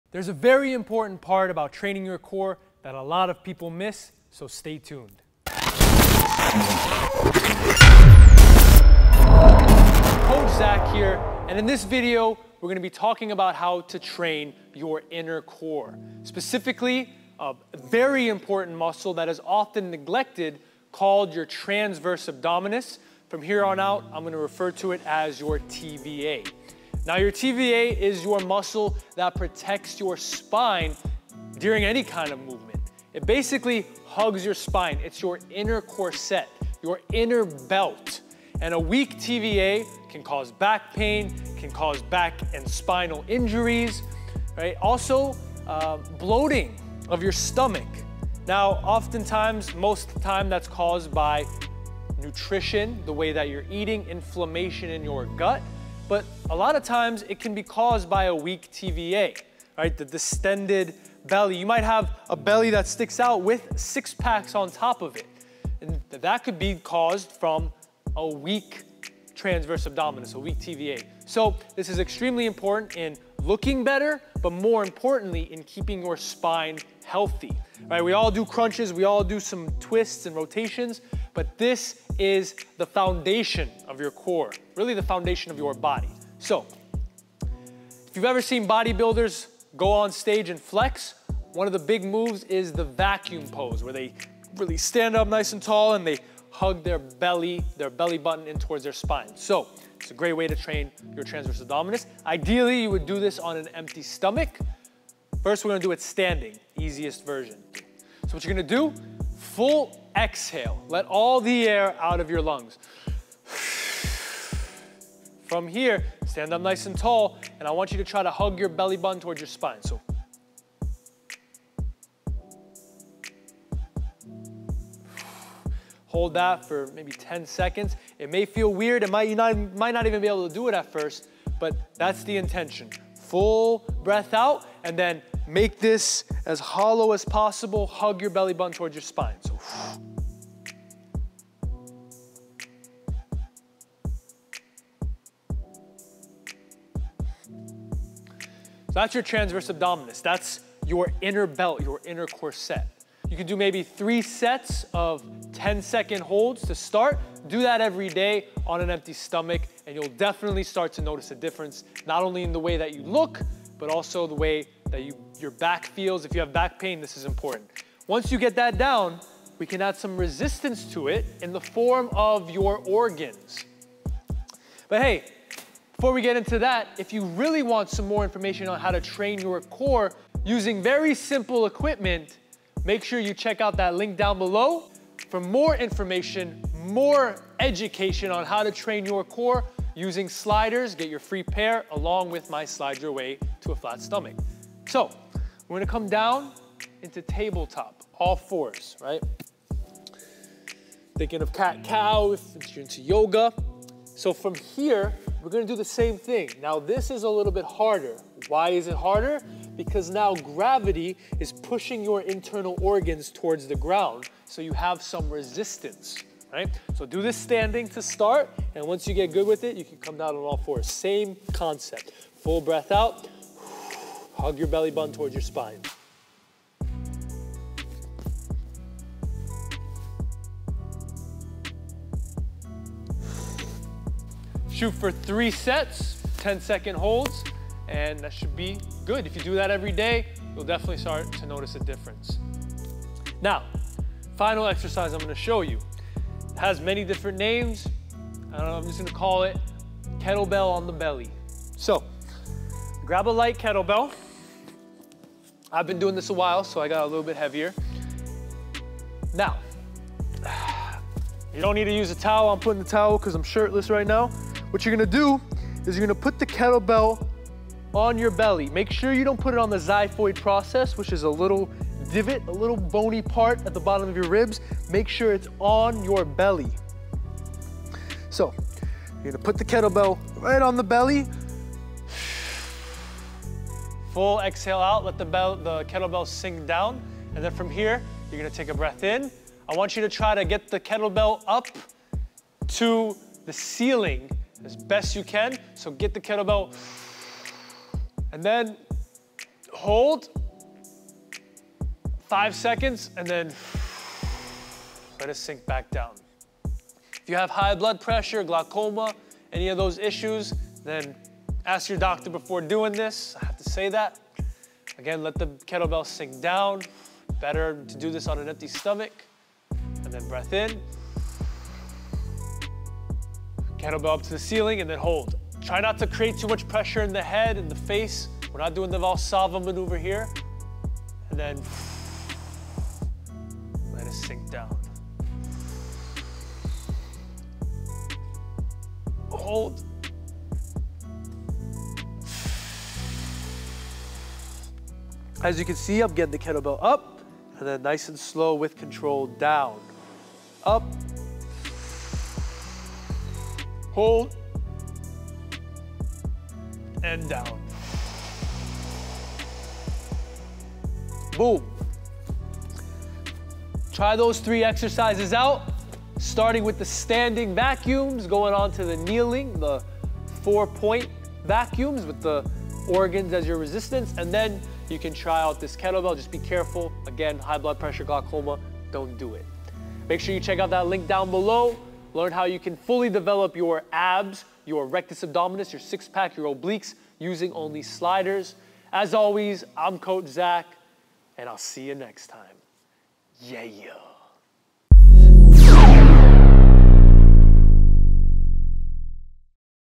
Inner belly muscle - How to train your Transverse Abdominus (TVA)
FREE Sliders, Slide your Way to a Flat Stomach!

How to Train your Inner Core - Specifically the Transverse Abdominus (TVA) Muscle

The transverse abdominis is located in the abdomen immediately inside of the internal oblique muscle. It is one of the innermost muscles of the abdomen.

Just like the other core muscles, the main roles of the transverse abdominis is to protect the internal organs by holding them in place, and also support the torso by maintaining abdominal wall tension that stabilizes the spine.

Coach Zach is going to show you three different methods of strengthening and tightening the TVA muscle. Not only will this change you're physique, and give you a slimmer tighter waist. This will also strengthen core stability muscles, and alleviate back pain and tightness.

Thank you 🙏 for watching, and Thank you 🙏 for your continued support of The Critical Bench Channel! 💪✊❤️